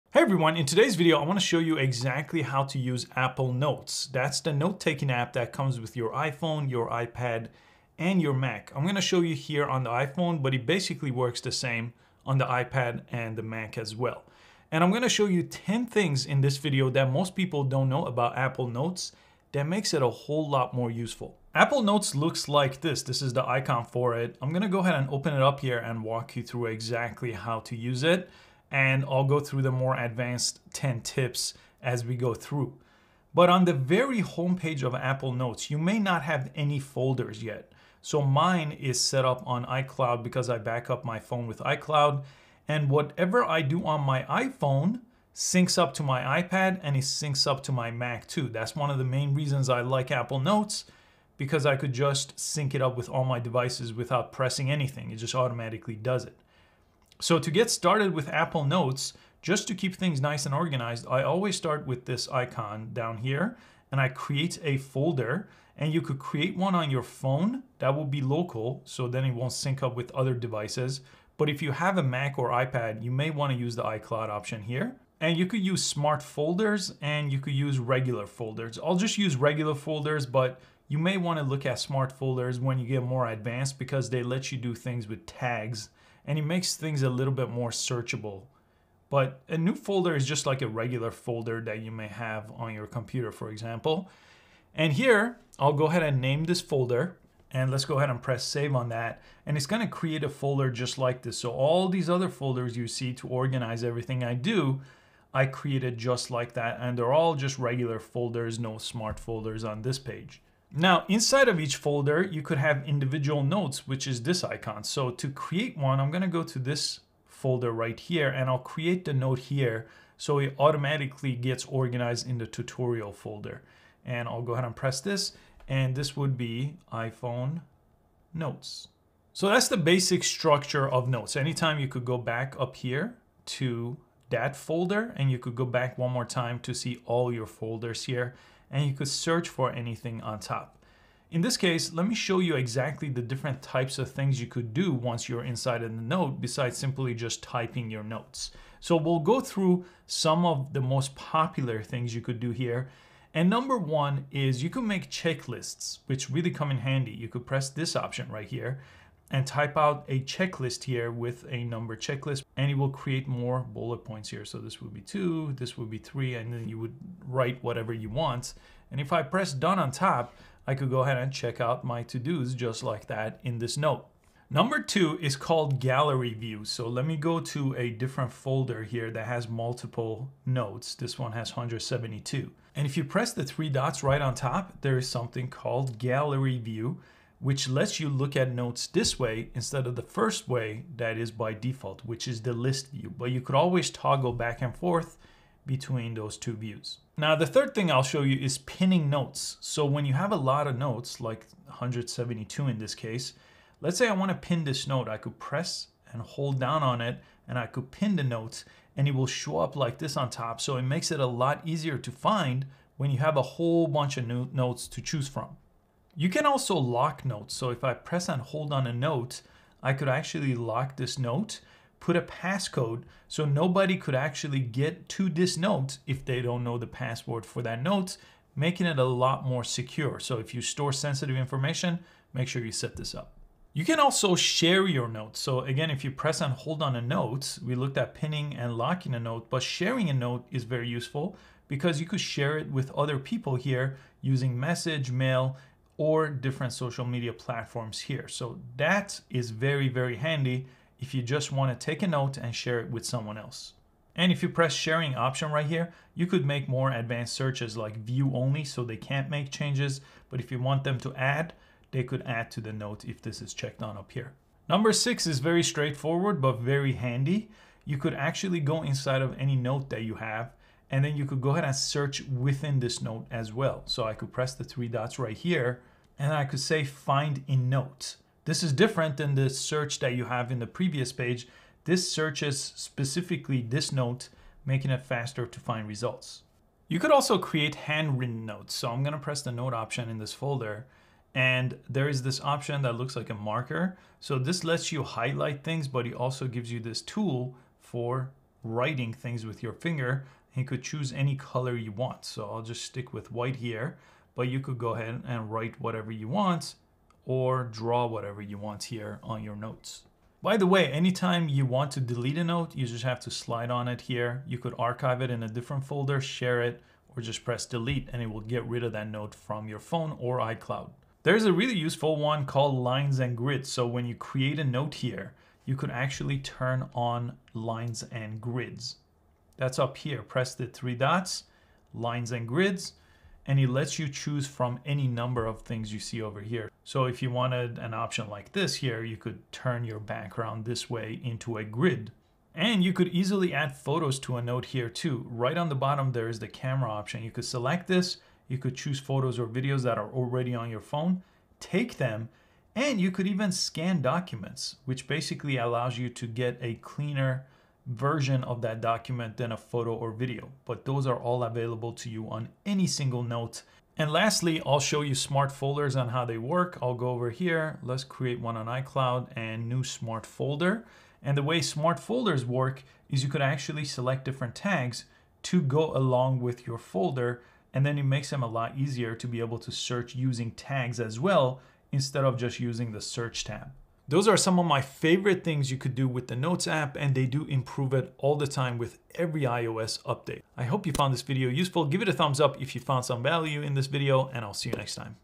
How to Use Apple Notes - 10 Tips Everyone Needs to Know
Do you take notes on your iPhone or iPad? With Apple Notes that comes with every iPhone or iPad, you can take notes that sync up with all your devices, organize those notes with folders or smart folders and use a variety of features that make the Apple Notes app the only notes app you need.

In this video, I'll show you how to use Apple notes and 10 very useful features that everyone taking notes on their iPhone, iPad, or Mac should know about including checklists, smart folders, and more.